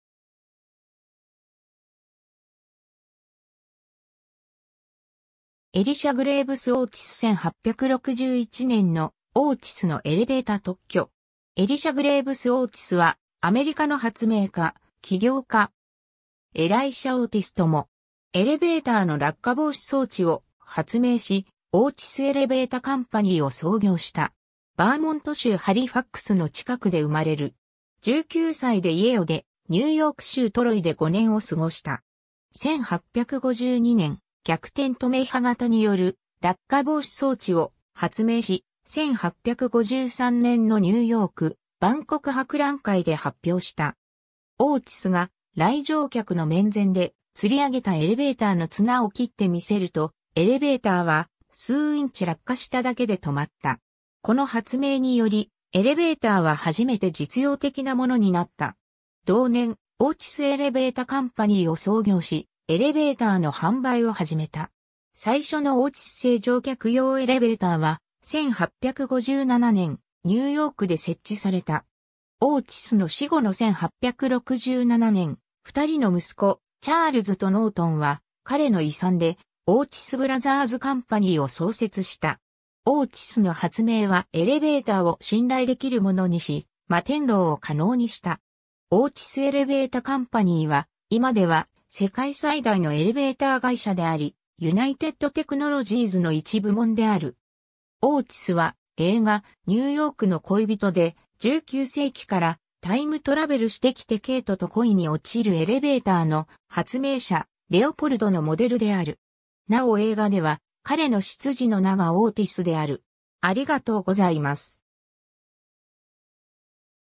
エリシャ・オーチス
オーチス・エレベータ・カンパニー
エリシャ・グレーブス・オーチス 1861年のオーチスのエレベータ特許。
 エリシャ・グレーブス・オーチス（Elisha Graves Otis、1811年8月3日 - 1861年4月8日）は、アメリカの発明家、企業家。
エライシャ・オーティスとも。
エレベーターの落下防止装置を発明し、オーチス・エレベータ・カンパニーを創業した。
 ヴァーモント州ハリファックスの近くで生まれる。
19歳で家を出、ニューヨーク州トロイで5年を過ごした。
 1852年、逆転止め歯形による落下防止装置（調速機、ガバナマシン）を発明し、1853年のニューヨーク万国博覧会で発表した。
オーチスが来場客の面前で、吊り上げたエレベーターの綱を切ってみせると、エレベーターは数インチ落下しただけで止まった。
この発明により、エレベーターは初めて実用的なものになった。
 同年、オーチス・エレベータ・カンパニーを創業し、エレベーターの販売を始めた。
最初のオーチス製乗客用エレベーターは、1857年ニューヨークで設置された。
オーチスの死後の1867年、2人の息子チャールズとノートンは、彼の遺産でオーチス・ブラザーズ・カンパニー (Otis Brothers & Co.) を創設した。
 オーチスの発明はエレベーターを信頼できるものにし、摩天楼を可能にした。
オーチス・エレベータ・カンパニーは、今では世界最大のエレベーター会社であり、ユナイテッド・テクノロジーズの一部門である。
 オーチスは、映画『ニューヨークの恋人』で、19世紀からタイムトラベルしてきてケイト（メグ・ライアン）と恋に落ちるエレベーターの発明者レオポルド（ヒュー・ジャックマン）のモデルである。
なお映画では、彼の執事の名がオーティスである。